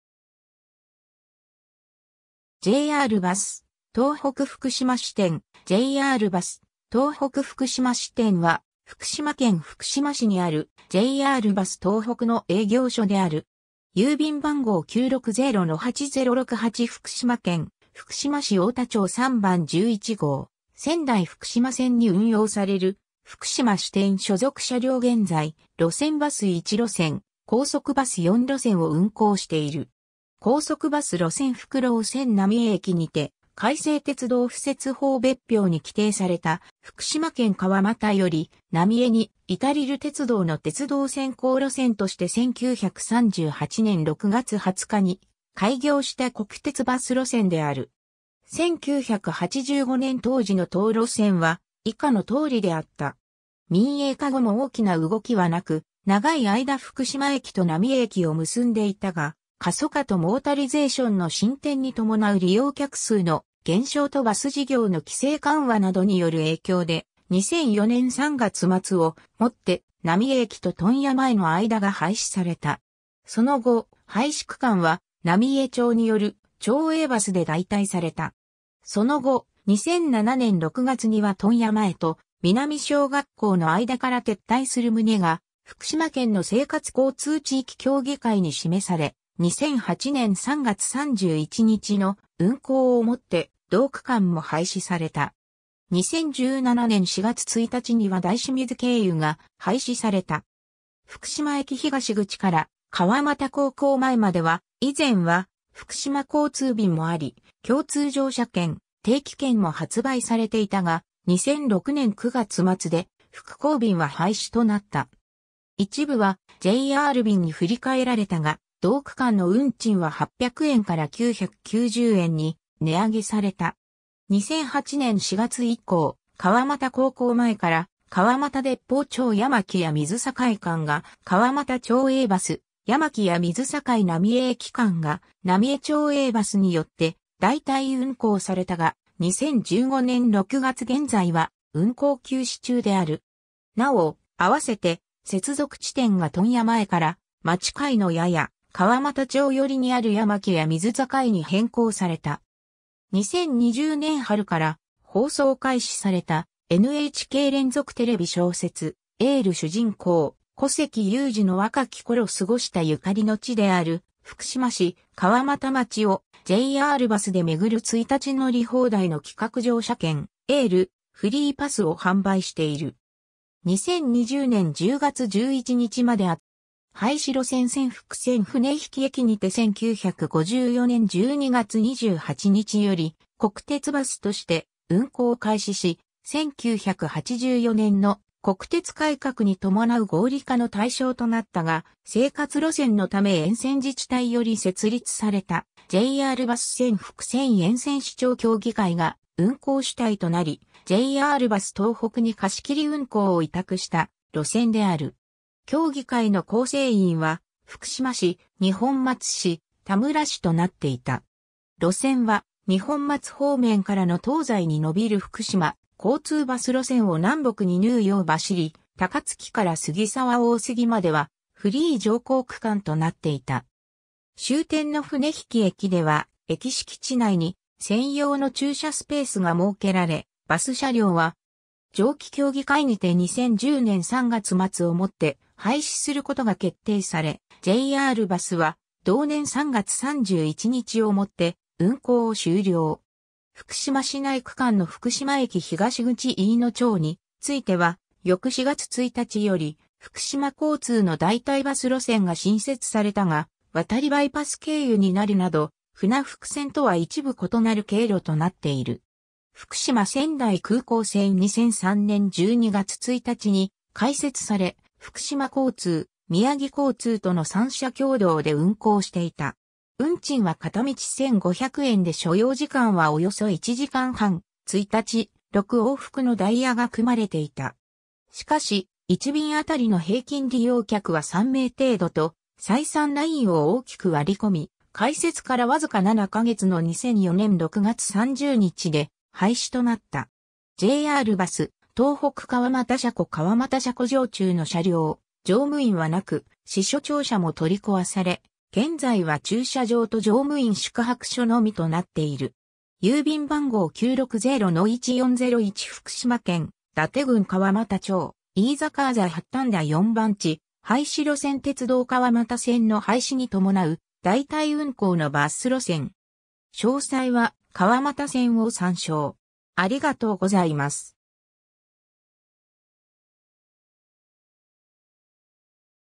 ジェイアールバス東北福島支店
ジェイアールバス東北福島支店 ジェイアールバス東北福島支店（ジェイアールバスとうほくふくしましてん）は、福島県福島市にあるジェイアールバス東北の営業所である。
 〒960-8068 福島県福島市太田町3番11号 仙台 - 福島線に運用される福島支店所属車両 現在、路線バス1路線、高速バス4路線を運行している（そのうち2路線は乗務員のみ担当）。
 高速バス路線 福浪線 （2004年2月）浪江駅にて 改正鉄道敷設法別表に規定された「福島県川俣ヨリ浪江ニ至ル鉄道」の鉄道先行路線として1938年（昭和13年）6月20日に開業した国鉄バス路線である。
 1985年当時の当路線は以下のとおりであった。
 民営化後も大きな動きはなく、長い間福島駅と浪江駅を結んでいたが、過疎化とモータリゼーションの進展に伴う利用客数の減少とバス事業の規制緩和などによる影響で、2004年3月末をもって浪江駅と問屋前の間が廃止された。
その後、廃止区間は、浪江町による町営バスで代替された。
その後、2007年6月には問屋前と南小学校 (11.7km) の間から撤退する旨が福島県の生活交通地域協議会に示され、2008年3月31日の運行をもって同区間も廃止された。
2017年4月1日には大清水経由（平日朝上り1本のみ運行）が廃止された。
 福島駅東口から川俣高校前までは、以前は福島交通便もあり、共通乗車券・定期券も発売されていたが、2006年9月末で福交便は廃止となった。
一部はJR便に振り替えられたが、同区間の運賃は800円から990円に値上げされた。
 2008年4月以降、川俣高校前 - 川俣鉄砲町 - 山木屋水境間が川俣町営バス（絹の里ふれあい号）、山木屋水境 - 浪江駅間が浪江町営バスによって代替運行されたが、2015年6月現在は運行休止中である。
なお、あわせて接続地点が問屋前から、町界のやや川俣町寄りにある山木屋水境に変更された。
 2020年春から放送開始されたNHK連続テレビ小説『エール』主人公・古関裕而の若き頃過ごしたゆかりの地である福島市・川俣町をJRバスで巡る一日乗り放題の企画乗車券「エール・フリーパス」を販売している。
2020年10月11日まで扱う予定である。
 廃止路線 船福線 （2005年10月）船引駅にて 1954年（昭和29年）12月28日より国鉄バスとして運行を開始し、1984年（昭和59年）の国鉄改革に伴う合理化の対象となったが、生活路線のため沿線自治体より設立されたJRバス船福線沿線市町協議会が運行主体となり、JRバス東北に貸切運行を委託した路線（廃止代替バス）である。
協議会の構成員は福島市・二本松市・田村市となっていた。
 路線は二本松方面からの東西に伸びる福島交通バス路線を南北に縫うよう走り、高槻から杉沢大杉まではフリー乗降区間となっていた。
終点の船引駅では、駅敷地内に専用の駐車スペースが設けられ、バス車両は折り返しまでの間そこで待機していた。
 上記協議会にて2010年（平成22年）3月末をもって廃止することが決定され、JRバスは同年3月31日をもって運行を終了。
福島市内区間の福島駅東口 - 飯野町については、翌4月1日より福島交通の代替バス路線（福島交通飯野町線）が新設されたが、渡利バイパス経由になるなど、船福線とは一部異なる経路となっている。
 福島 - 仙台空港線（高速バス） 2003年12月1日に開設され、福島交通、宮城交通との3社共同で運行していた。
運賃は片道1,500円で所要時間はおよそ1時間半、1日6往復のダイヤが組まれていた。
しかし、1便あたりの平均利用客は3名程度と採算ラインを大きく割り込み、開設からわずか7か月の2004年6月30日で廃止となった。
 JRバス東北川俣車庫（2009年10月撮影） 川俣車庫常駐の車両・乗務員はなく、支所庁舎も取り壊され、現在は駐車場と乗務員宿泊所のみとなっている。
 〒960-1401 福島県伊達郡川俣町飯坂字八反田4番地 廃止路線 鉄道川俣線の廃止に伴う代替運行のバス路線。
詳細は川俣線を参照。